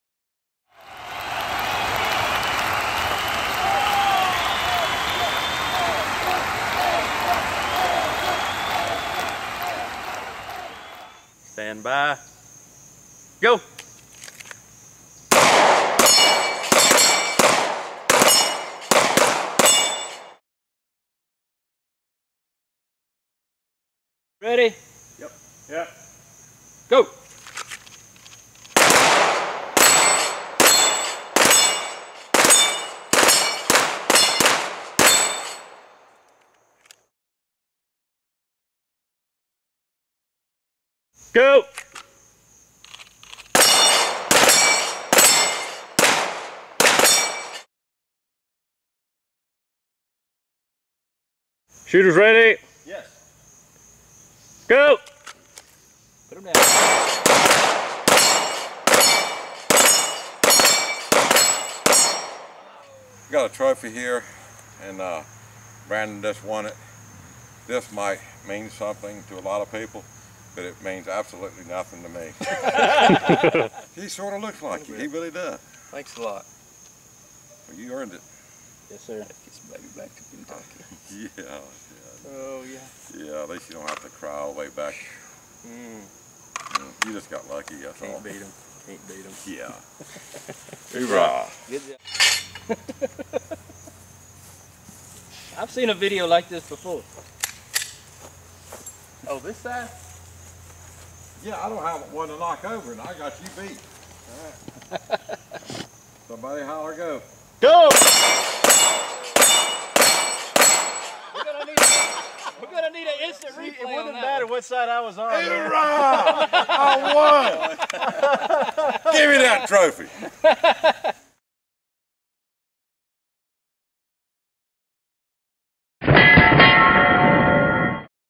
Shooting Dueling Tree Target - King of the Tree
BDH King of The Tree. Friendly competition between friends.